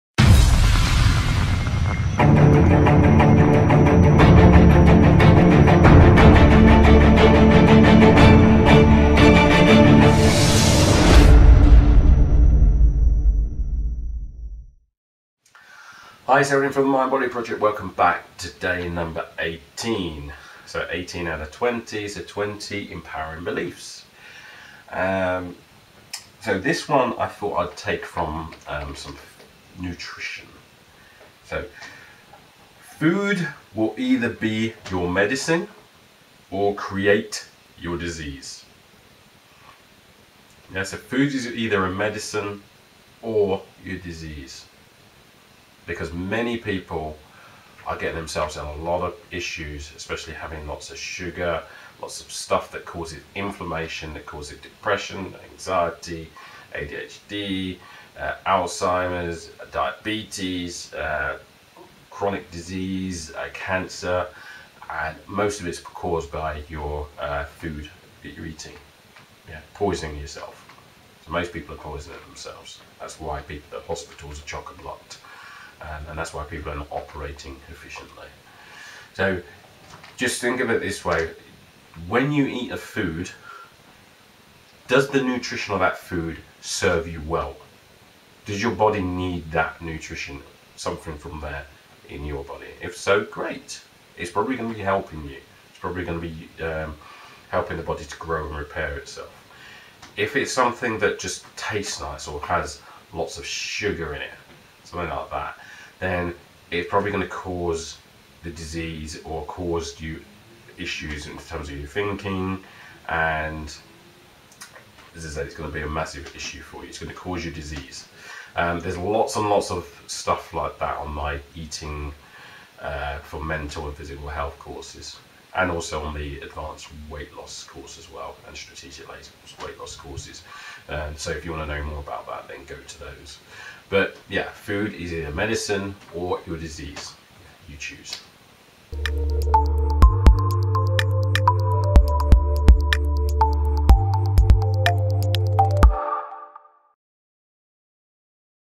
20 Empowering Beliefs - 18/20 - "Food is either your medicine or will create your disease!"
20 Empowering Beliefs

18/20 - "Food is either your medicine or will create your disease!"

As mentioned on numerous of my healthy eating and weight loss courses, your lifestyle is the cause of over 90% of disease and can be reversed by aggressive nutritional changes by a professional. Small changes help slow down the issue but is not enough to reverse these conditions because you've essentially been poisoning yourself for years.

Look up nutritional therapists or functional medicine doctors for help with disease so they can give you the help you need. A functional medicine doctor can do what your doctor does but can also help with nutrition, meditation, physical activity, and strategies around removing toxins which your doctor either can't or doesn't have time to tell you!